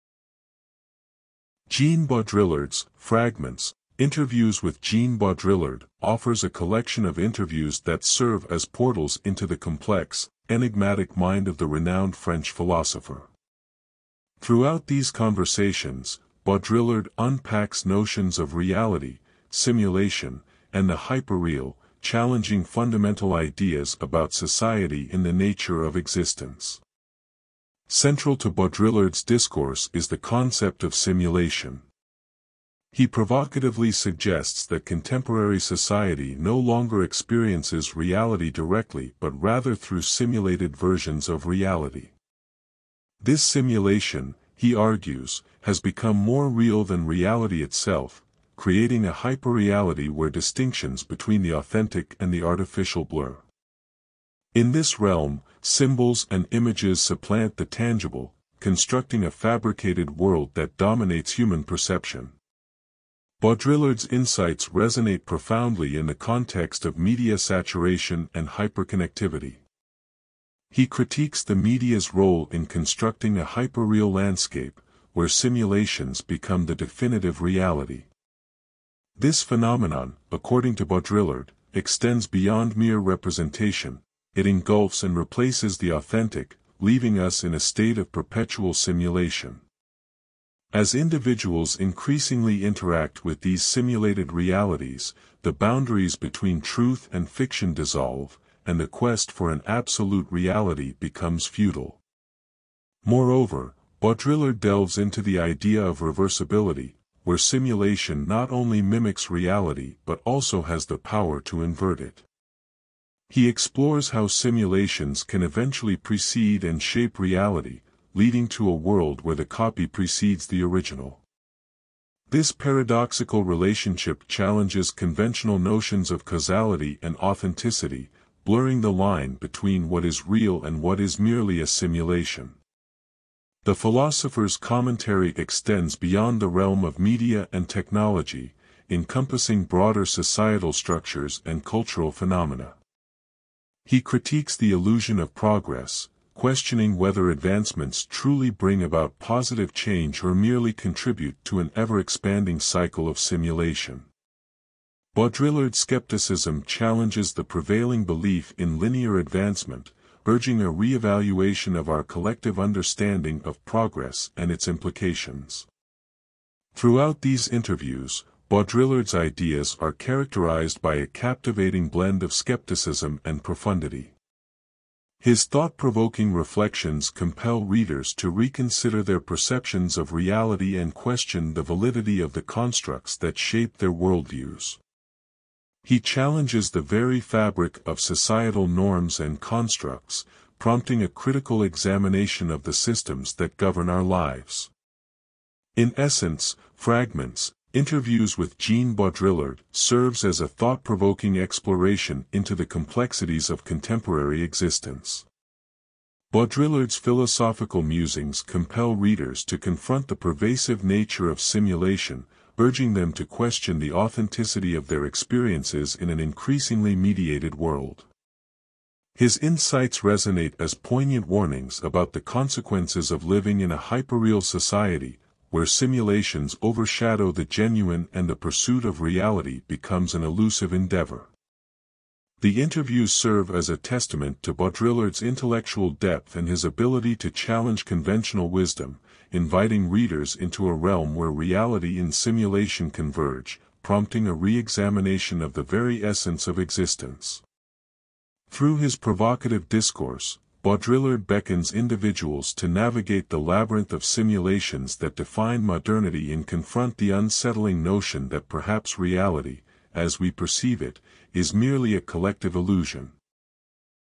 "Fragments" By Jean Baudrillard
Jean Baudrillard's "Fragments: Interviews with Jean Baudrillard" offers a collection of interviews that serve as portals into the complex, enigmatic mind of the renowned French philosopher. Throughout these conversations, Baudrillard unpacks notions of reality, simulation, and the hyperreal, challenging fundamental ideas about society and the nature of existence.Central to Baudrillard's discourse is the concept of simulation. He provocatively suggests that contemporary society no longer experiences reality directly but rather through simulated versions of reality. This simulation, he argues, has become more real than reality itself, creating a hyperreality where distinctions between the authentic and the artificial blur. In this realm, symbols and images supplant the tangible, constructing a fabricated world that dominates human perception.Baudrillard's insights resonate profoundly in the context of media saturation and hyperconnectivity. He critiques the media's role in constructing a hyperreal landscape, where simulations become the definitive reality. This phenomenon, according to Baudrillard, extends beyond mere representation; it engulfs and replaces the authentic, leaving us in a state of perpetual simulation. As individuals increasingly interact with these simulated realities, the boundaries between truth and fiction dissolve, and the quest for an absolute reality becomes futile.Moreover, Baudrillard delves into the idea of reversibility, where simulation not only mimics reality but also has the power to invert it. He explores how simulations can eventually precede and shape reality, leading to a world where the copy precedes the original. This paradoxical relationship challenges conventional notions of causality and authenticity, blurring the line between what is real and what is merely a simulation.The philosopher's commentary extends beyond the realm of media and technology, encompassing broader societal structures and cultural phenomena. He critiques the illusion of progress, questioning whether advancements truly bring about positive change or merely contribute to an ever-expanding cycle of simulation. Baudrillard's skepticism challenges the prevailing belief in linear advancement, urging a reevaluation of our collective understanding of progress and its implications.Throughout these interviews, Baudrillard's ideas are characterized by a captivating blend of skepticism and profundity. His thought-provoking reflections compel readers to reconsider their perceptions of reality and question the validity of the constructs that shape their worldviews. He challenges the very fabric of societal norms and constructs, prompting a critical examination of the systems that govern our lives.In essence, "Fragments: Interviews with Jean Baudrillard" serves as a thought-provoking exploration into the complexities of contemporary existence. Baudrillard's philosophical musings compel readers to confront the pervasive nature of simulation, urging them to question the authenticity of their experiences in an increasingly mediated world. His insights resonate as poignant warnings about the consequences of living in a hyperreal society, where simulations overshadow the genuine and the pursuit of reality becomes an elusive endeavor.The interviews serve as a testament to Baudrillard's intellectual depth and his ability to challenge conventional wisdom, inviting readers into a realm where reality and simulation converge, prompting a reexamination of the very essence of existence. Through his provocative discourse, Baudrillard beckons individuals to navigate the labyrinth of simulations that define modernity and confront the unsettling notion that perhaps reality, as we perceive it, is merely a collective illusion.